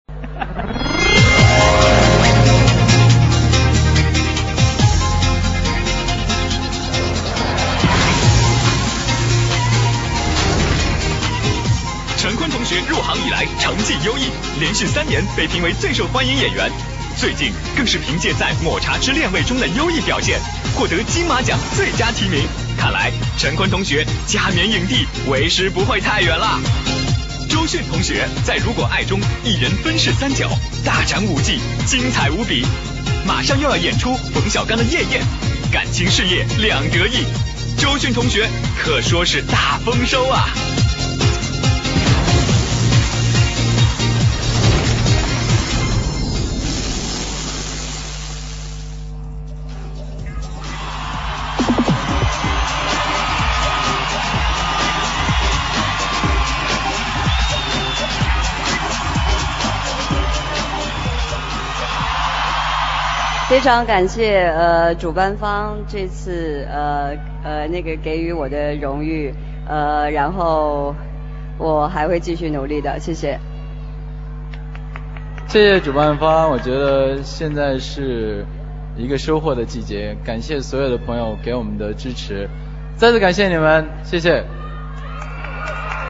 Zhou Xun in the 2nd MTV Super Concert Awards
in the 2nd MTV Super Concert Awards:
Zhou Xun received "The most charismatic style award" in Mainland China.
Zhou Xun and Chen Kun won "the most charismatic style partners" awards in the silver screen.
Credit: cpy @ zhouxun.tv